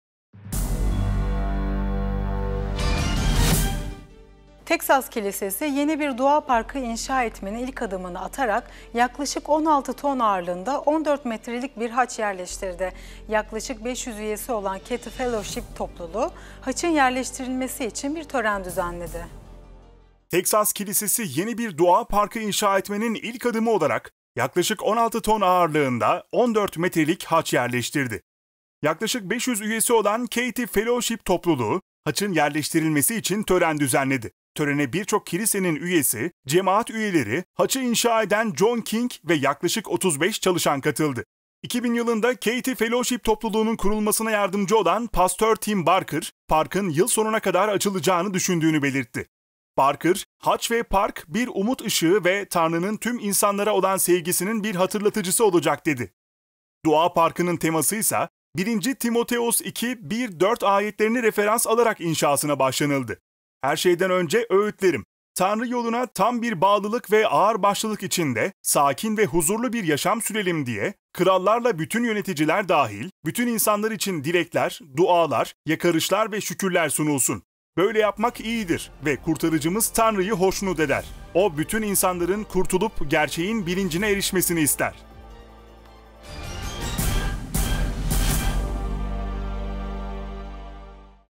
Teksas Kilisesi, Yeni Dua Parkına 14 Metrelik Haç Yerleştirdi | 16.09.2020
Teksas kilisesi, yeni bir dua parkı inşa etmenin ilk adımı olarak, yaklaşık 16 ton ağırlığında 14 metrelik haç yerleştirdi.

Yaklaşık 500 üyesi olan Katy Fellowship Topluluğu, devasa haçın yerleştirilmesi için tören düzenledi. Törene birçok kilise üyesi, haçı inşa eden John King ve yaklaşık 35 çalışan katıldı.
‘‘Haç, tesisimizin yanına dua parkı inşa edilmesinin ilk adımı olarak yerleştirildi’’ diyen 2000 yılında Katy Fellowship Topluluğu’nun kurulmasına yardımcı olan Pastör Tim Barker, haçın zamanla kırmızımsı bir renge dönüşmesi beklenen korten çelikten yapıldığını ve 280 çivinin kullanıldığını belirtti.
Barker, ‘‘Her çivinin cemaatimizdeki aileleri temsil etmesi için dua edildi. Haç ve dua parkı için ilham kaynağı Teksas, Kerrville’deki sanatçı ve mimar olan Max Greiner Jr.’dan geldi’’ dedi.

Diğer aşama ise, haçın dibinde bir vaftiz alanı ve amfitiyatro oluşturulması olacak. Ardından yürüyüş parkurları ve tabelalar eklenecek. Barker, ‘‘Üçüncü aşama kurtuluş, şifa, aile, uyanış, kaleler ve uluslar olmak üzere yedi dua noktasını birbirine bağlayan yürüyüş yollarını kapsayacak. Kutsal Kitap’ın 66 bölümünden, 66 kutsal yazı parkın her yerindeki tabelalarda yerini alacak.  Beyaz bir kaya bahçesi, dua isteklerinin bir kayaya yazılabileceği ve insanların dua etmesi için bahçeye bırakabileceği şifa ve kale noktalarını çevreleyecek’’ ifadelerinde bulundu.

Parkın yıl sonuna kadar açılacağını düşünen Barker, ‘‘Haç ve park, bir umut ışığı ve Tanrı’nın tüm insanlara olan sevgisinin bir hatırlatıcısı olacak’’ dedi.
Dua parkının teması ise 1. Timoteos 2:1-4 bölümünde yazılı: ‘‘Her şeyden önce öğütlerim: Tanrı yoluna tam bir bağlılık ve ağırbaşlılık içinde sakin ve huzurlu bir yaşam sürelim diye, krallarla bütün yöneticiler dâhil, bütün insanlar için dilekler, dualar, yakarışlar ve şükürler sunulsun. Böyle yapmak iyidir ve Kurtarıcımız Tanrı’yı hoşnut eder. O bütün insanların kurtulup gerçeğin bilincine erişmesini ister.’’

2018’de Teksas eyaletinin güneyindeki Corpus Christi şehrinde inşasına başlanan haç tamamlandığında 70 metre uzunluğunda ve 30 metre genişliğinde olup, Kuzey Amerika’nın en büyük haçı olacak. Tamamen bağışlarla desteklenen projeyi Güney Teksas’taki Abundant Life Fellowship Kilisesi pastörü Rick Milby denetliyor. (CP)